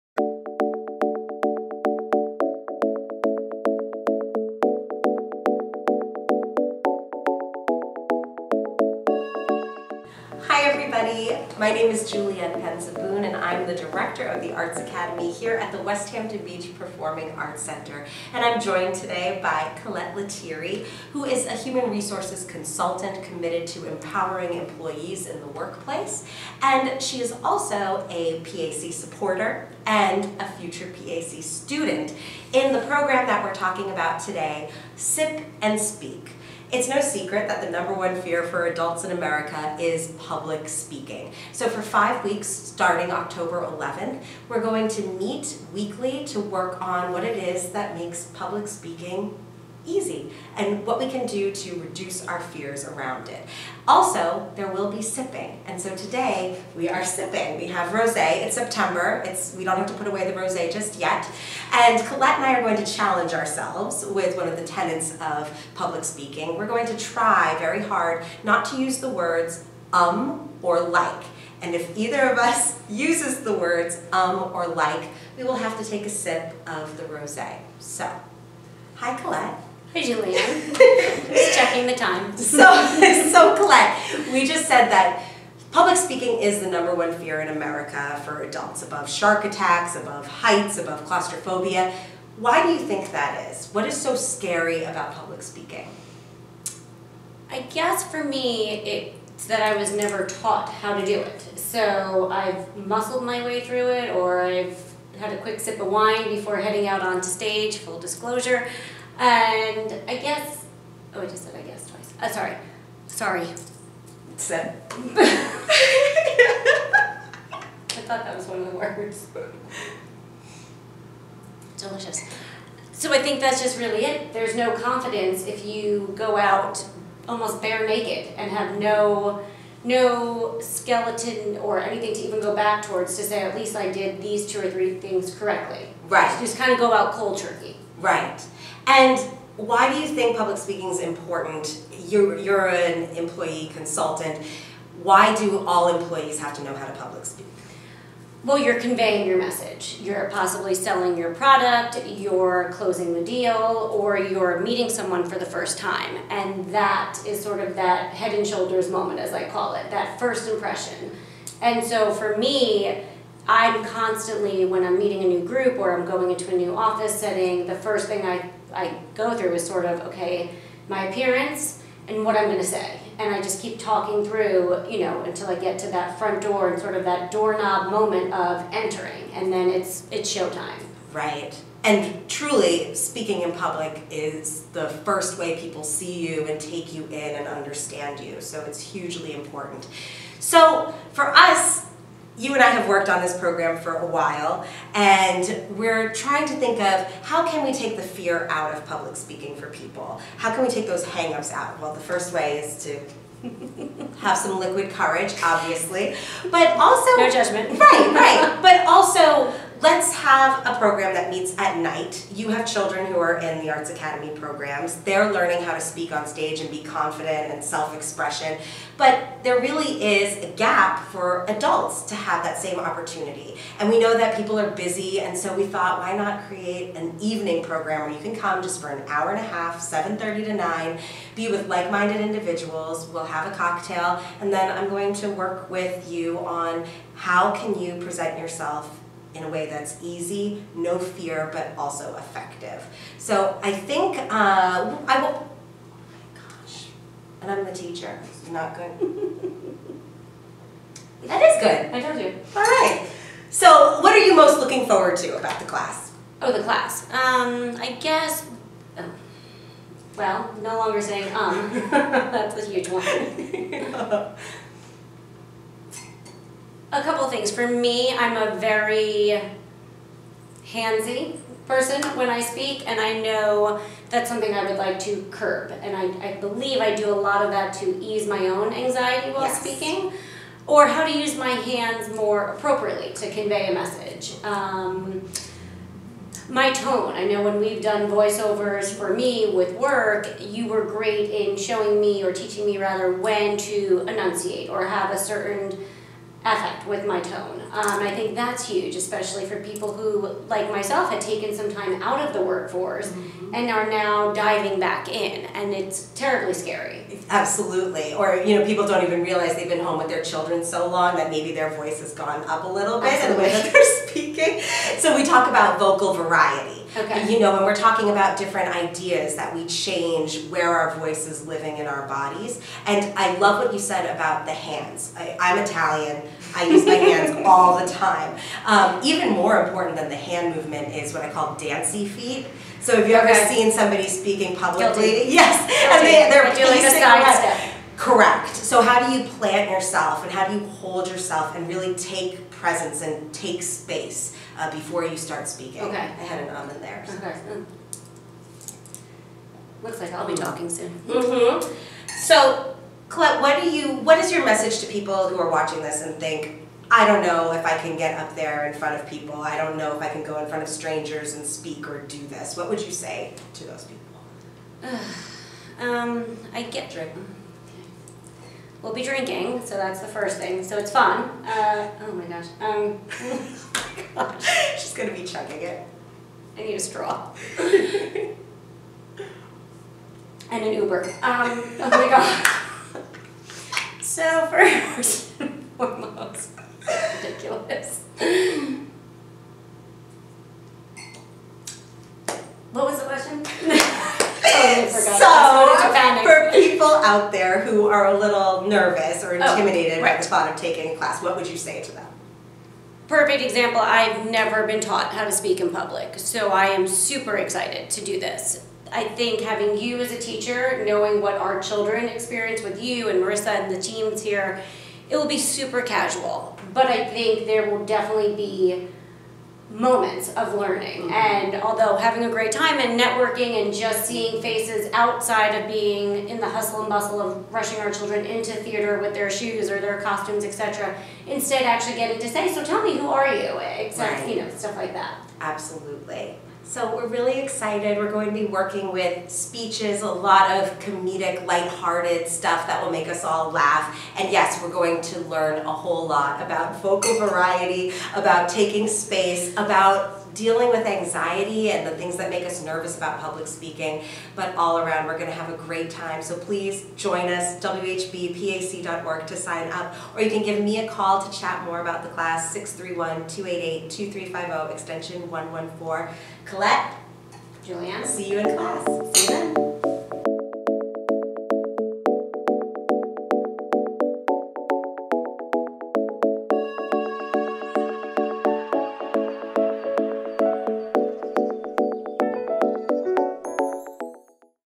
WHBPAC Introduces Sip & Speak!
The WHBPAC's Director of Arts Education chats with Colette Lettieri on the upcoming adult public speaking course, Sip & Speak!